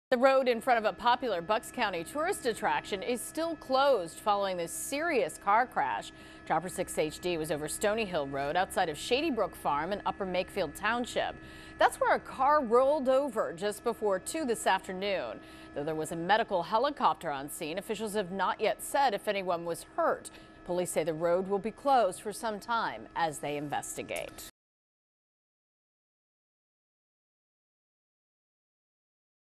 Police investigate crash in Lower Makefield Twp.
Police are investigating a two-vehicle crash in Bucks County that left multiple people injured.